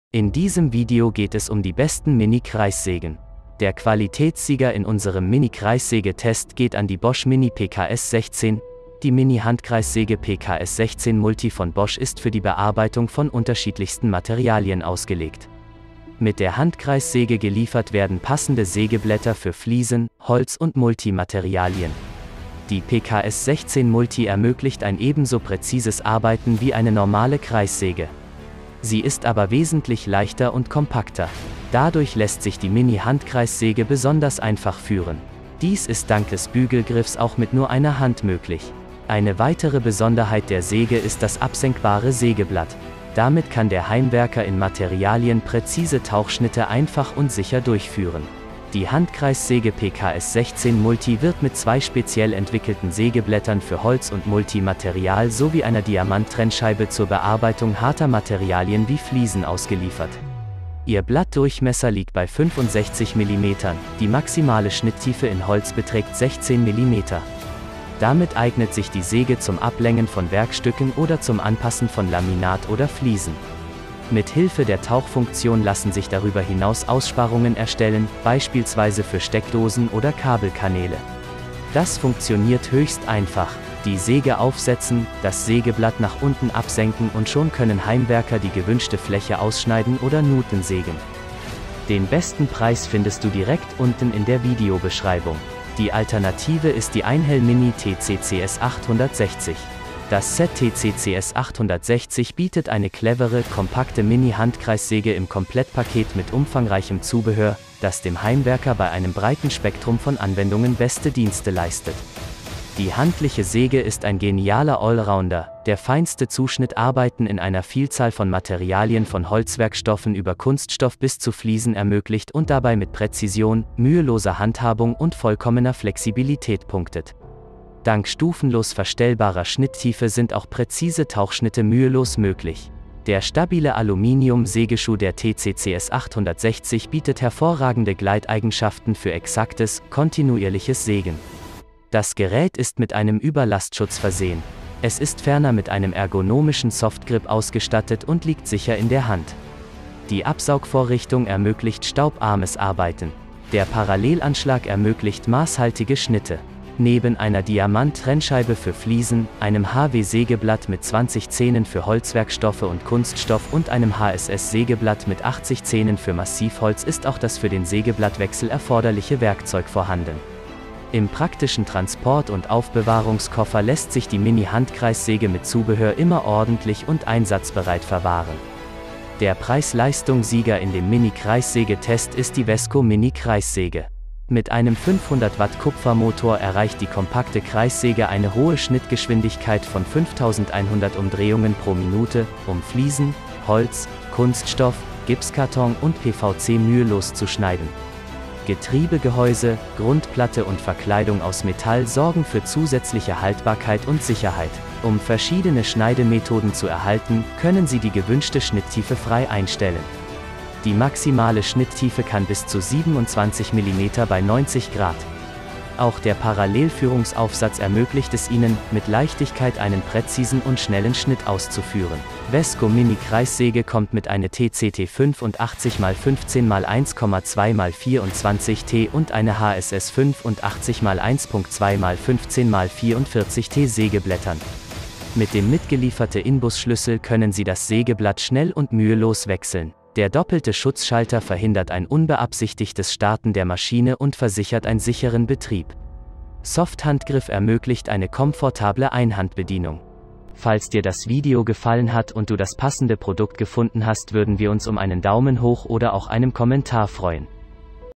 ▶ Mini Kreissäge Test 2025 | 3 Beste Mini Kreissägen
Der Qualitätssieger in unserem Mini Kreissäge Test geht an die Bosch Mini PKS 16.

Die Mini Handkreissäge PKS 16 Multi von Bosch ist für die Bearbeitung von unterschiedlichsten Materialien ausgelegt. Mit der Handkreissäge geliefert werden passende Sägeblätter für Fliesen, Holz und Multi Materialien. Die PKS 16 Multi ermöglicht ein ebenso präzises Arbeiten wie eine normale Kreissäge. Sie ist aber wesentlich leichter und kompakter. Dadurch lässt sich die Mini Handkreissäge besonders einfach führen. Dies ist dank des Bügelgriffs auch mit nur einer Hand möglich. Eine weitere Besonderheit der Säge ist das absenkbare Sägeblatt. Damit kann der Heimwerker in Materialien präzise Tauchschnitte einfach und sicher durchführen.

Die Handkreissäge PKS 16 Multi wird mit zwei speziell entwickelten Sägeblättern für Holz und Multi Material sowie einer Diamant Trennscheibe zur Bearbeitung harter Materialien wie Fliesen ausgeliefert. Ihr Blatt Durchmesser liegt bei 65 Millimetern, die maximale Schnitttiefe in Holz beträgt 16 Millimeter. Damit eignet sich die Säge zum Ablängen von Werkstücken oder zum Anpassen von Laminat oder Fliesen. Mit Hilfe der Tauchfunktion lassen sich darüber hinaus Aussparungen erstellen, beispielsweise für Steckdosen oder Kabelkanäle. Das funktioniert höchst einfach. Die Säge aufsetzen, das Sägeblatt nach unten absenken und schon können Heimwerker die gewünschte Fläche ausschneiden oder Nuten sägen.

Die alternative ist die Einhell Mini TC CS 860.

Das Set TC CS 860 bietet eine clevere, kompakte Mini Handkreissäge im Komplettpaket mit umfangreichem Zubehör, das dem Heimwerker bei einem breiten Spektrum von Anwendungen beste Dienste leistet. Die handliche Säge ist ein genialer Allrounder, der feinste Zuschnitt arbeiten in einer Vielzahl von Materialien von Holzwerkstoffen über Kunststoff bis zu Fliesen ermöglicht und dabei mit Präzision, müheloser Handhabung und vollkommener Flexibilität punktet. Dank stufenlos verstellbarer Schnitttiefe sind auch präzise Tauchschnitte mühelos möglich. Der stabile Aluminium Sägeschuh der TC CS 860 bietet hervorragende Gleiteigenschaften für exaktes, kontinuierliches Sägen. Das Gerät ist mit einem Überlastschutz versehen. Es ist ferner mit einem ergonomischen Softgrip ausgestattet und liegt sicher in der Hand. Die Absaugvorrichtung ermöglicht staubarmes Arbeiten. Der Parallelanschlag ermöglicht maßhaltige Schnitte. Neben einer Diamant Trennscheibe für Fliesen, einem HW Sägeblatt mit 20 Zähnen für Holzwerkstoffe und Kunststoff und einem HSS Sägeblatt mit 80 Zähnen für Massivholz ist auch das für den Sägeblattwechsel erforderliche Werkzeug vorhanden. Im praktischen Transport und Aufbewahrungskoffer lässt sich die Mini Handkreissäge mit Zubehör immer ordentlich und einsatzbereit verwahren.
Der Preis Leistung Sieger in dem Mini Kreissäge Test ist die Wesco Mini Kreissäge.

Mit einem 500 Watt Kupfermotor erreicht die kompakte Kreissäge eine hohe Schnittgeschwindigkeit von 5100 Umdrehungen pro Minute, um Fliesen, Holz, Kunststoff, Gipskarton und PVC mühelos zu schneiden. Getriebegehäuse, Grundplatte und Verkleidung aus Metall sorgen für zusätzliche Haltbarkeit und Sicherheit.
Um verschiedene Schneidemethoden zu erhalten, können Sie die gewünschte Schnitttiefe frei einstellen. Die maximale Schnitttiefe kann bis zu 27 Millimeter bei 90 Grad. Auch der Parallelführungsaufsatz ermöglicht es Ihnen, mit Leichtigkeit einen präzisen und schnellen Schnitt auszuführen.
Wesco Mini Kreissäge kommt mit eine TCT 85 mal 15 mal 1,2 mal 24 T und eine HSS 85 mal 1.2 mal 15 mal 44T Sägeblättern. Mit dem mitgelieferte Inbusschlüssel können Sie das Sägeblatt schnell und mühelos wechseln.
Der doppelte Schutzschalter verhindert ein unbeabsichtigtes Starten der Maschine und versichert ein sicheren Betrieb. Softhandgriff ermöglicht eine komfortable Einhandbedienung.